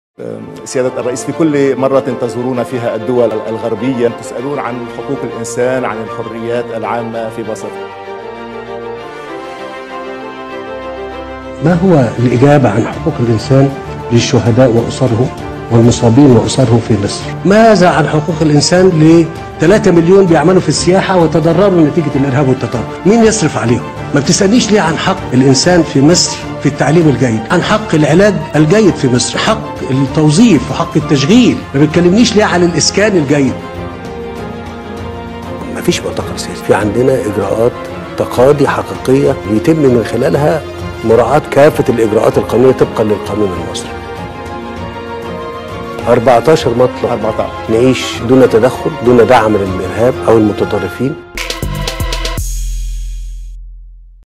رد تاريخي من السيسي عن حقوق الإنسان في مصر ورأيه في أزمة قطر
عنوان الفيديو | رد تاريخي من السيسي عن حقوق الإنسان في مصر ورأيه في أزمة قطر